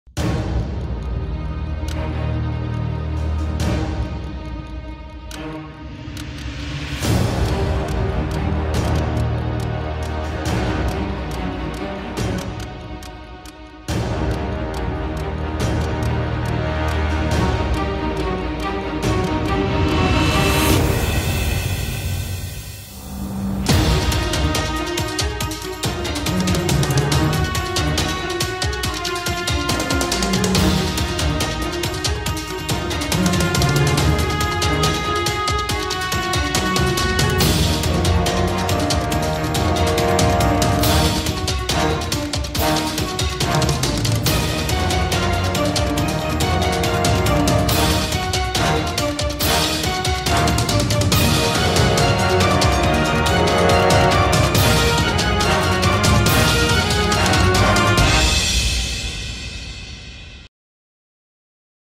Kayaking Championship Teaser -Malabar River Fest
Kayaking championship, as part of Malabar river festival. Monsoon tourism events in Kerala. The video is released as an official teaser for the event. Sourced from Social media.